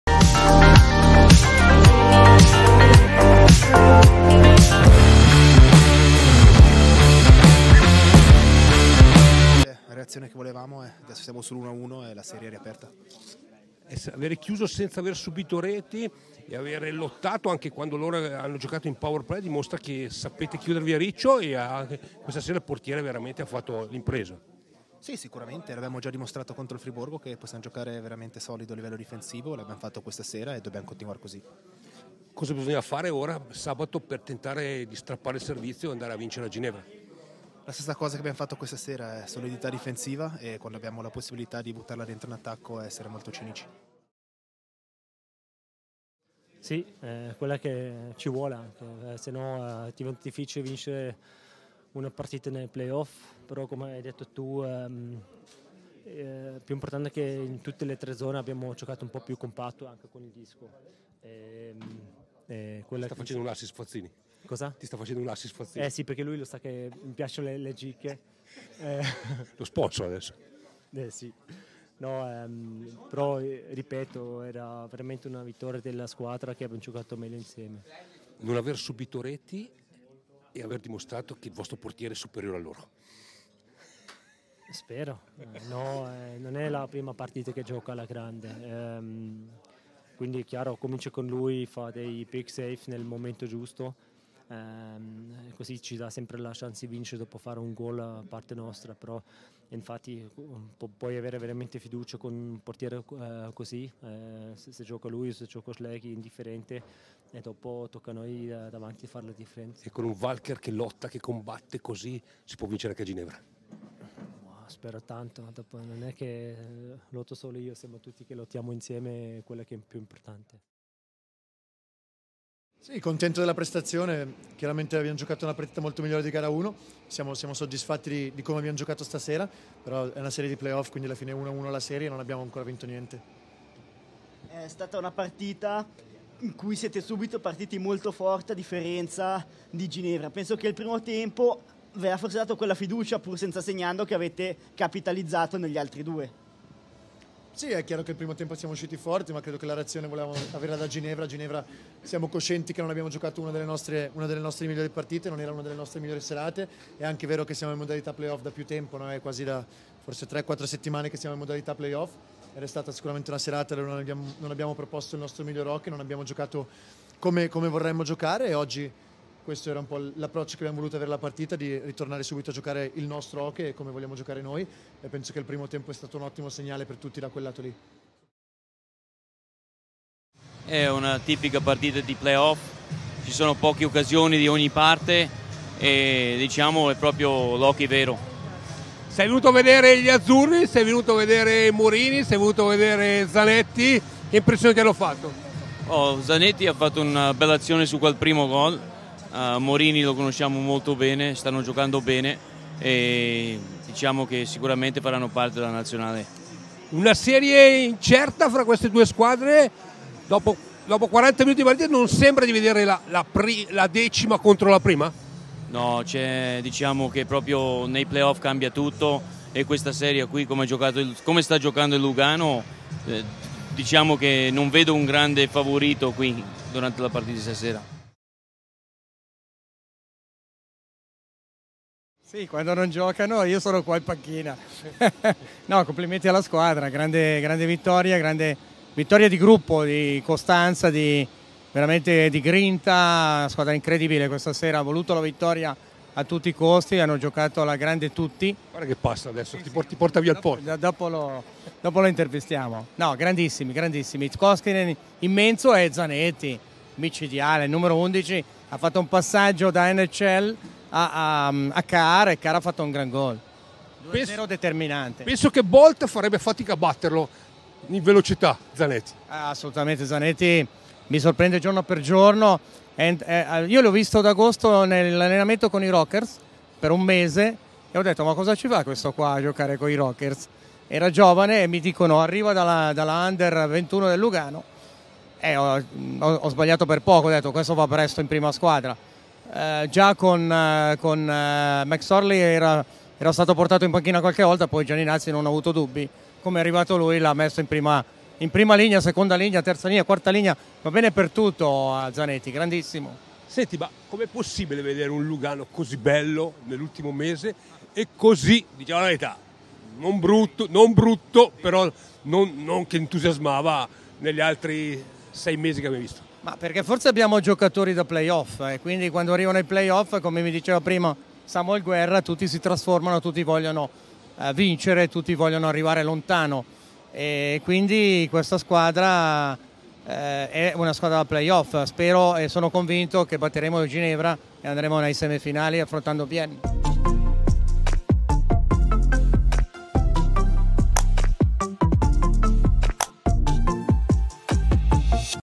HC Lugano vs HC Geneve Servette - National League - Interviste
Interviste a cura di Roberto Vedani della 2^Gara Quarti Finale Giornata Playoff Campionato Swiss National Hockey League  - Swiss Ice Hockey tra Hockey Club Lugano ed HC Geneve -Servette disputata Giovedì 16 Marzo 2023 presso la Corner Arena di Lugano (Canton Ticino - Svizzera) , interviste ai giocatori Luca Fazzini e Julian Walker Marco, all'Head Coach Luca Gianinazzi, al giornalista Doriano Barsega , e all'Assistent Coach della Nazionale Italiana Luca Scandella